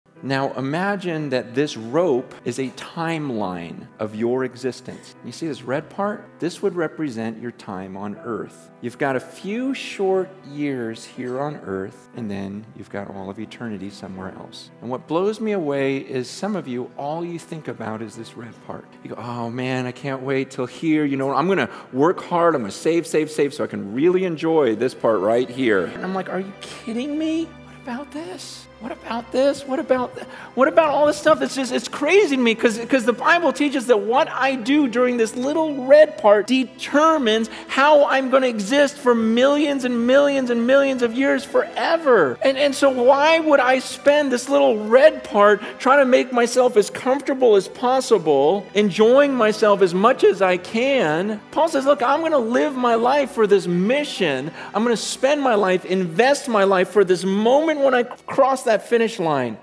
Matthew 6:19-20
Matthew 6:19-20 - “Do not lay up for yourselves treasures on earth, where  moth and rust  destroy and where thieves break in and steal, but lay up for yourselves treasures in heaven, where neither moth nor rust destroys and where thieves do not break in and steal."

Original Video - Francis Chan: Living Eternally (The Rope Sermon) - Crazy Love